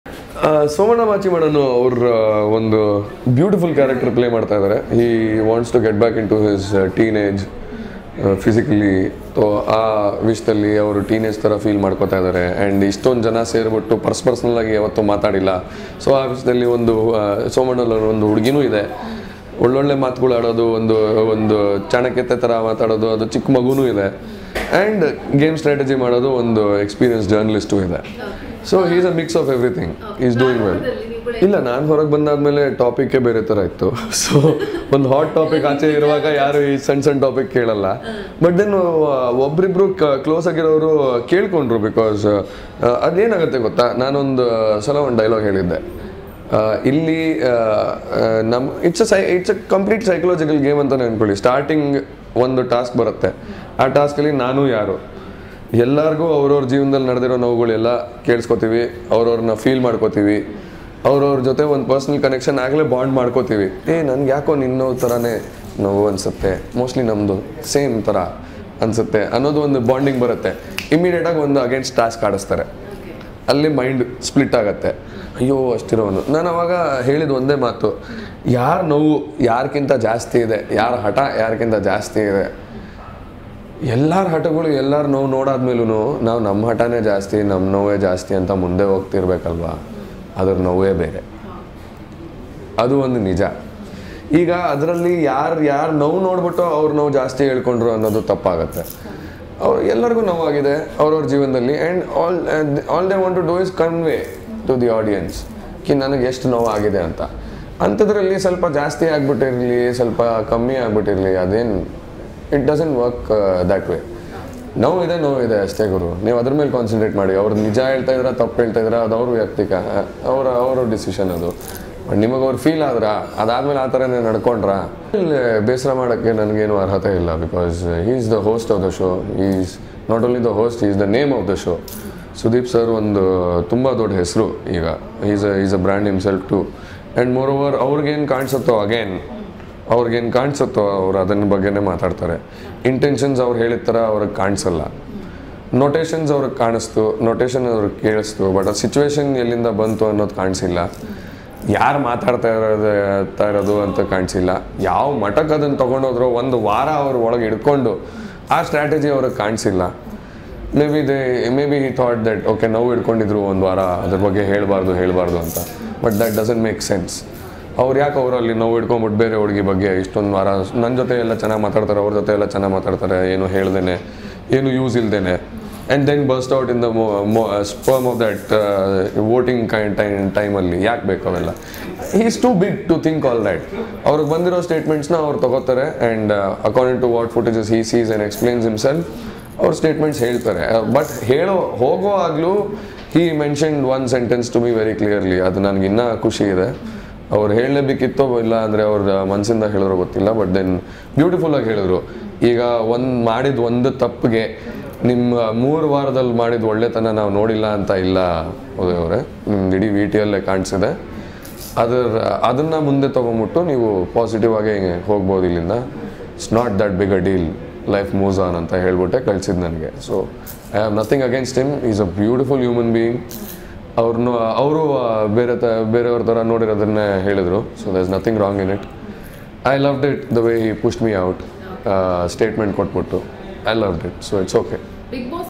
Uday Surya || ಸೋಮಣ್ಣ ಚೆನ್ನಾಗಿ ಆಟವಾಡ್ತಿದ್ದಾರೆ || Big Boss || Zee Kannada News
ಸುದೀಪ್‌ ಬಂದಿರೋ ಸ್ಟೇಟ್‌ಮೆಂಟ್‌ ತೆಗೆದುಕೊಂಡು ಮಾತಾಡ್ತಾರೆ ಎಂದು ಬಿಗ್‌ ಬಾಸ್‌ ಸ್ಪರ್ಧಿ ಉದಯ್‌ ಸೂರ್ಯ ಹೇಳಿದ್ದಾರೆ.. ಇದು ಜೀ ಕನ್ನಡ ನ್ಯೂಸ್‌ಗೆ ಬಿಗ್‌ ಬಾಸ್‌ ಸ್ಪರ್ಧಿ ಉದಯ್‌ ಸೂರ್ಯ ನೀಡಿದ ವಿಶೇಷ ಸಂದರ್ಶನ..